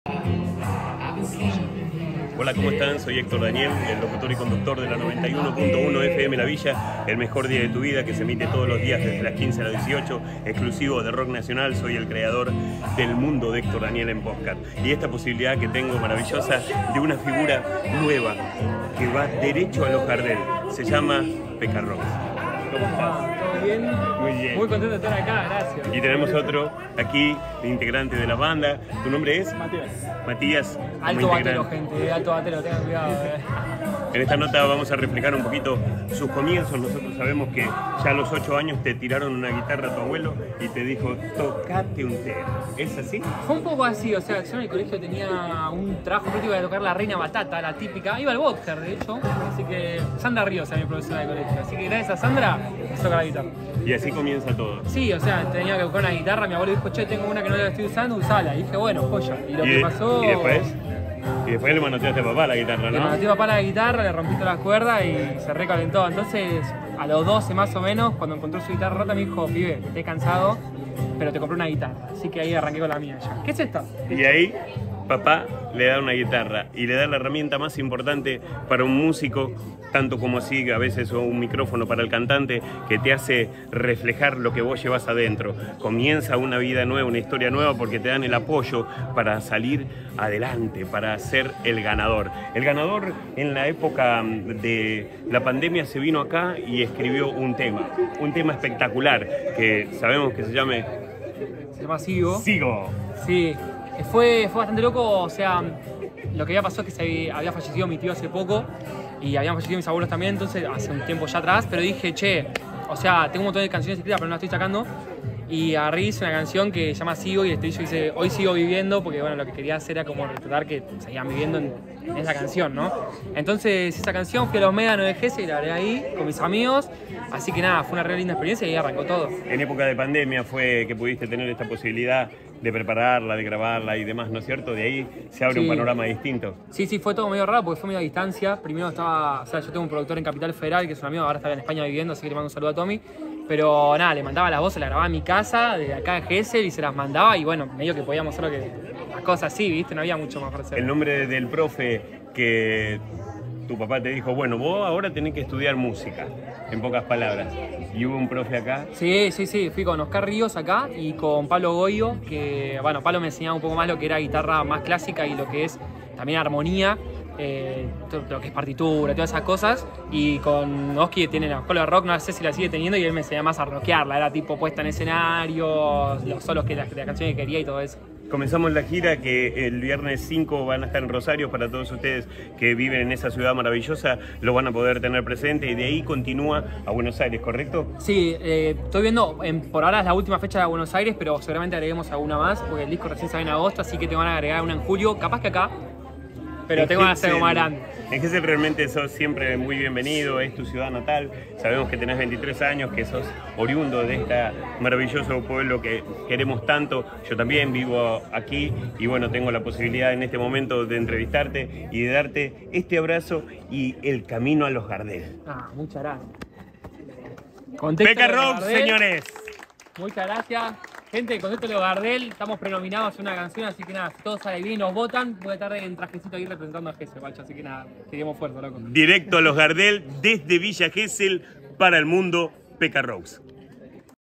LA HISTORIA DE PEKA ROUX Episodio 10
Hoy te Presentamos la Historia de Peka Roux un Geselino que está Prenominado a los Premios Gardel

Contáctame!

Gmail de negocios: hectordanielcontactbusiness